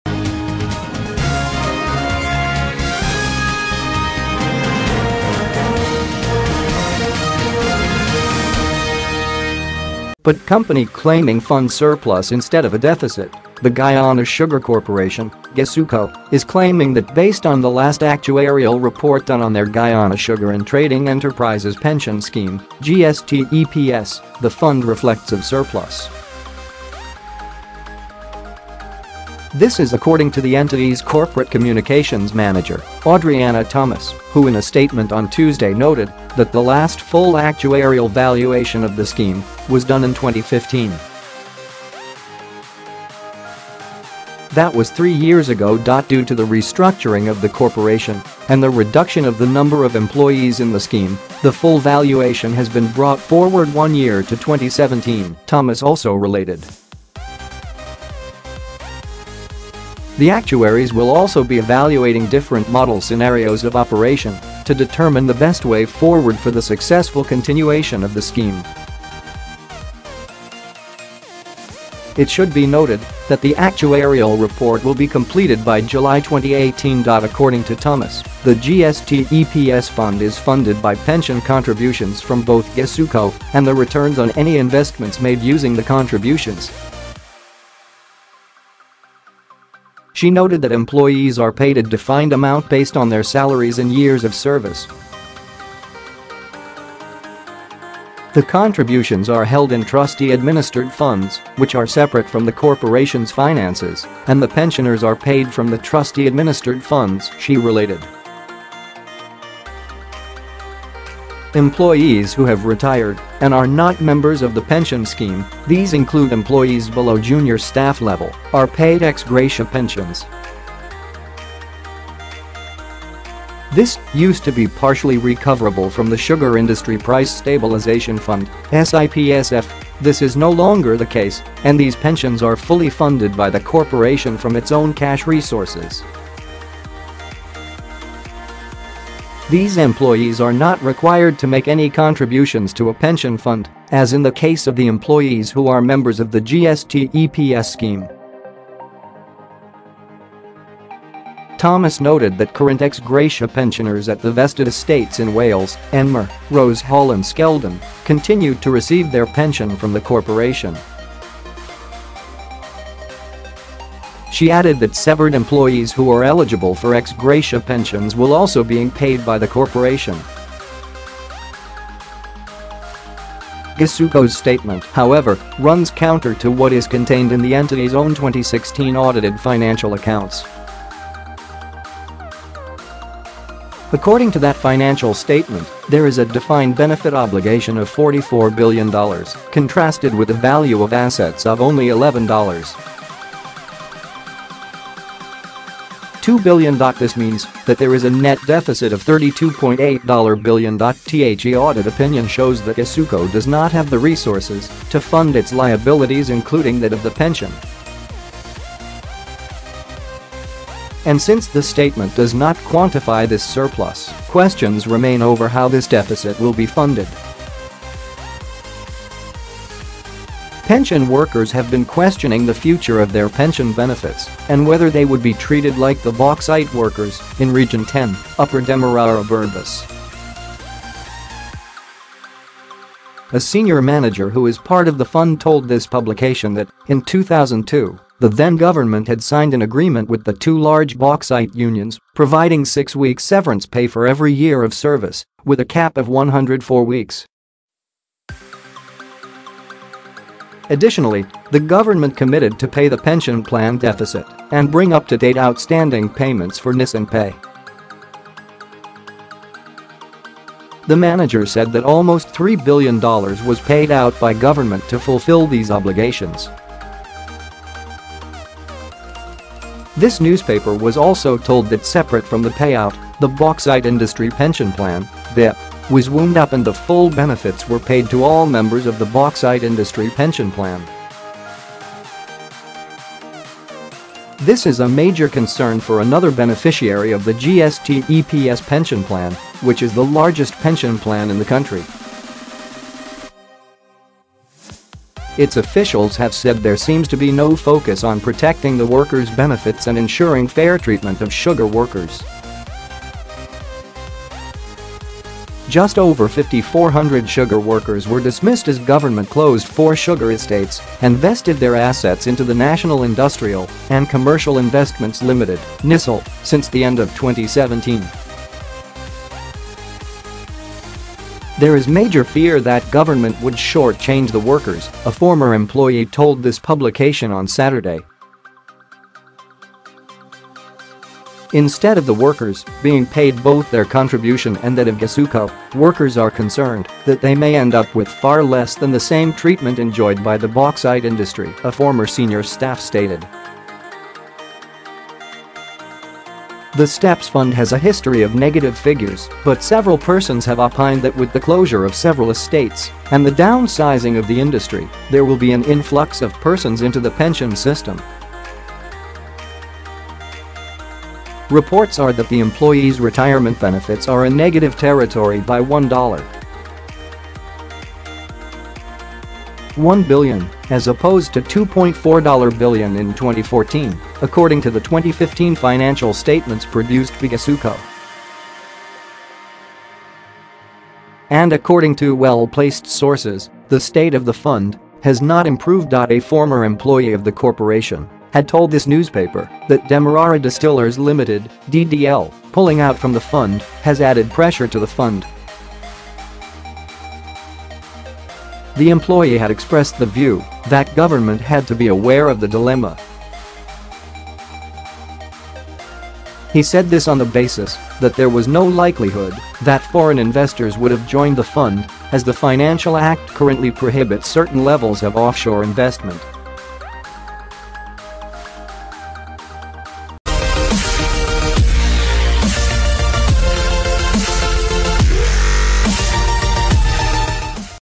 GUYANA NEWS-2016 audited accounts show $32.8B net deficit
…but company claiming fund surplus Instead of a deficit, the Guyana Sugar Corporation (GuySuCo) is claiming that based on the last actuarial report done on their Guyana Sugar and Trading Enterprises Pension Scheme (GSTEPS), the fund reflects a surplus.This is according to the entity’s Corporate Communications Manager, Audreyanna Thomas, who in a statement on Tuesday noted that the last full actuarial valuation of the scheme was done in 2015.That was three years ago.“Due to the restructuring of the Corporation and the reduction of the number of employees in the scheme, the full valuation has been brought forward one year to 2017,” Thomas also related.“The actuaries will also be evaluating different models/scenarios of operation to determine the best way forward for the successful continuation of the scheme.It should be noted that the actuarial report will be completed by July 2018.”According to Thomas, the GSTEPS fund is funded by pension contributions from both GuySuCo and the returns on any investments made using the contributions.She noted that employees are paid a defined amount based on their salaries and years of service.“The contributions are held in trustee administered funds, which are separate from the Corporation’s finances and the pensioners are paid from the trustee administered funds,” she related.“Employees who have retired and are not members of the pension scheme, these include employees below junior staff level, are paid ex-gratia pensions.”“(This) used to be partially recoverable from the Sugar Industry Price Stabilisation Fund (SIPSF); this is no longer the case and these pensions are fully funded by the Corporation from its own cash resources.These employees are not required to make any contributions to a pension fund as in the case of the employees who are members of the GSTEPS scheme.”Thomas noted that current ex-gratia pensioners at the vested estates in Wales, Enmore, Rose Hall and Skeldon, continue to receive their pension from the Corporation.She added that severed employees who are eligible for ex-gratia pensions will also being paid by the Corporation.GuySuCo’s statement, however, runs counter to what is contained in the entity’s own 2016 audited financial accounts.According to that financial statement, there is a defined benefit obligation of $44 billion, contrasted with a value of assets of only $11.2billion.This means that there is a net deficit of $32.8billion.The audit opinion shows that GuySuCo does not have the resources to fund its liabilities including that of the pension.And since the statement does not quantify this ‘surplus’, questions remain over how this deficit will be funded.Pension Workers have been questioning the future of their pension benefits and whether they would be treated like the bauxite workers, in Region 10 (Upper Demerara-Berbice).A senior manager who is part of the fund told this publication that, in 2002, the then Government had signed an agreement with the two large bauxite unions, prov